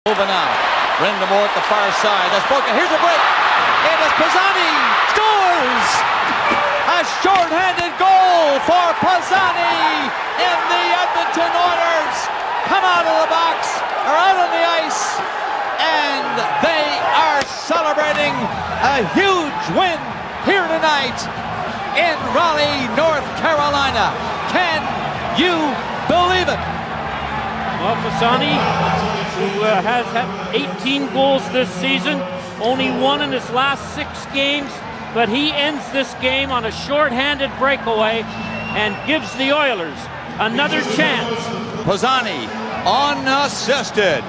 Pisani scores overtime goal in 2006 Stanley Cup final
Fernando Pisani makes history for the Edmonton Oilers as he becomes the first player ever to score a shorthanded overtime goal in a Stanley Cup final game in Game 5 against the Carolina Hurricanes.